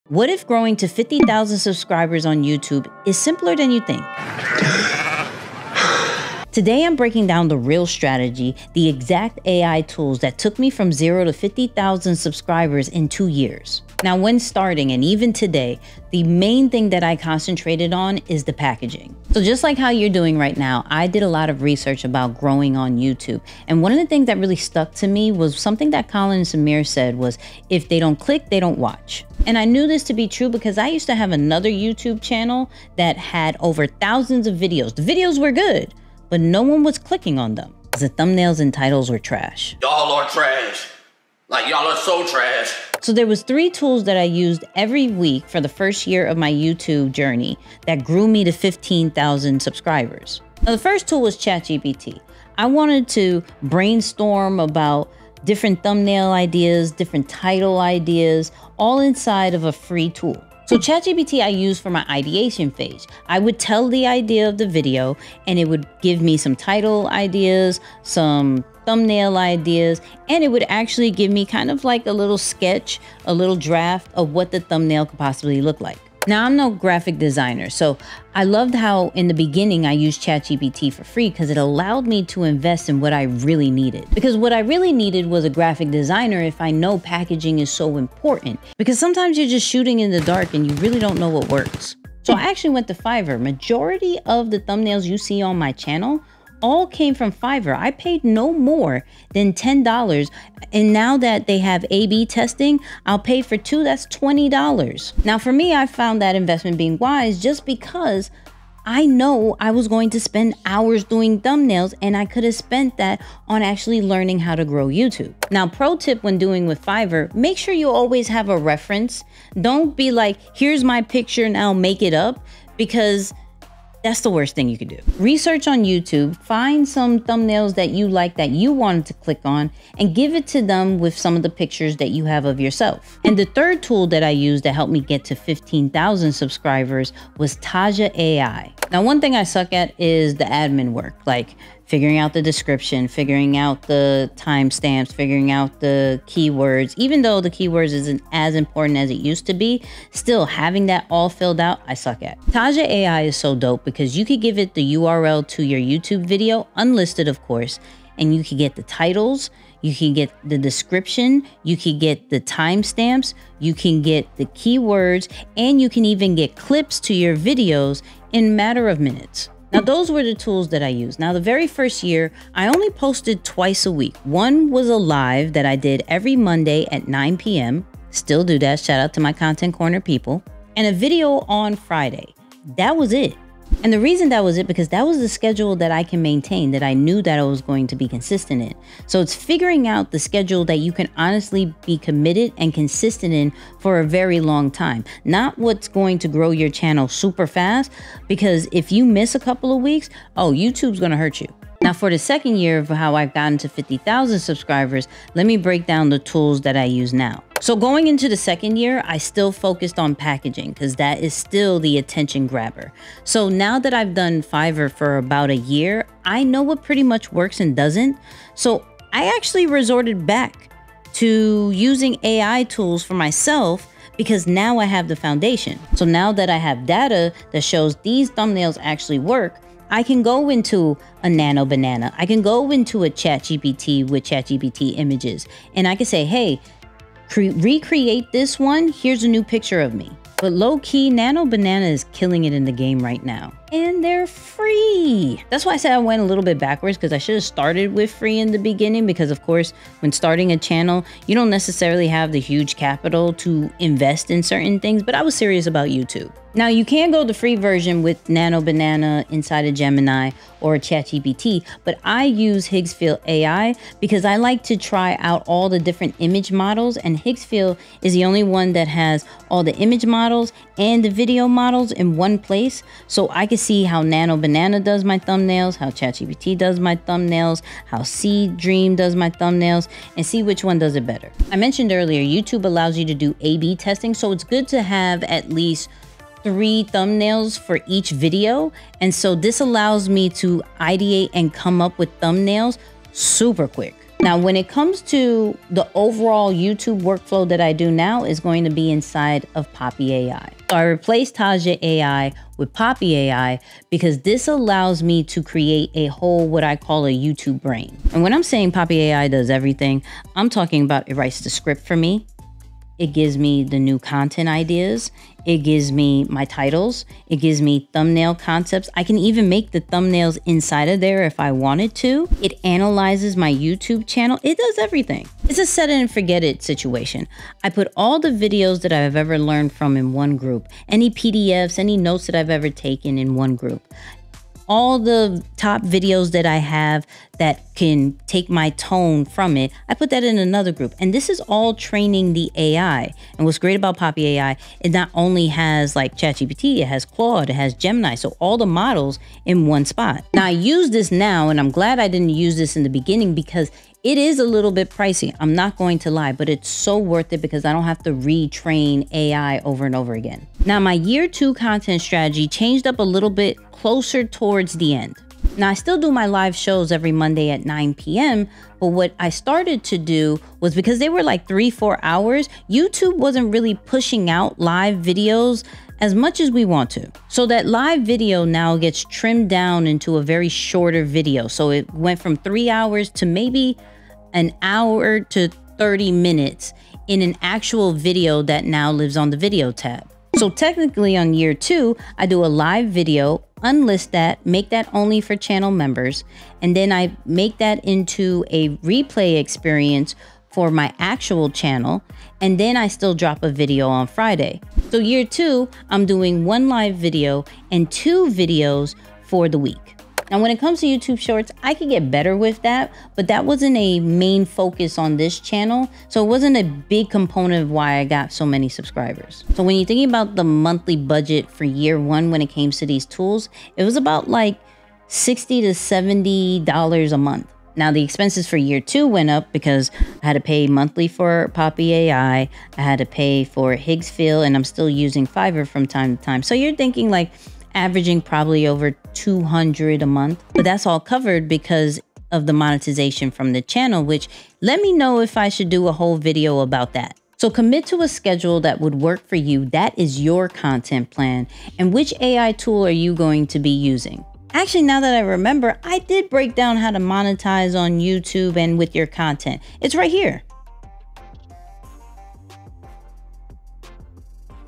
She Let AI Grow Her Channel... And It Gained 50K Subs
I let AI take over my YouTube channel growth and the results shocked me. In this video, I break down exactly how I used AI tools to go from zero to 50,000 subscribers. I’ll show you my full workflow, the tools that actually worked, and the surprising lessons I learned along the way.

If you want to grow your channel faster and smarter in 2026, this is the video you need.

Join Lions Behavior 🦁 Test AI. Build habits. Create fearlessly with no judgement

More about the video:

00:00 Intro
00:43 Tools I used from 0-15k subscribers
03:23 Tools I used from 15k-50k subscribers
06:04 Content Strategy used to get 50k subcribers
7:01 Budgets